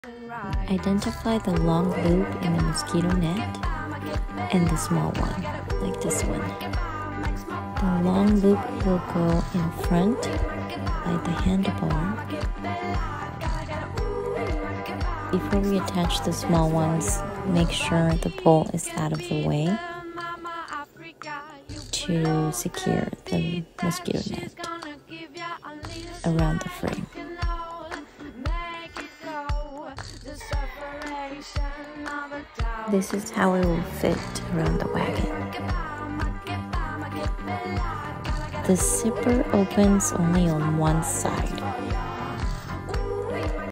How to install the Mosquito Net - Wonderfold Luxe Wagons
How to install the mosquito  net for the Wonderfold stroller wagons two seater and four seater.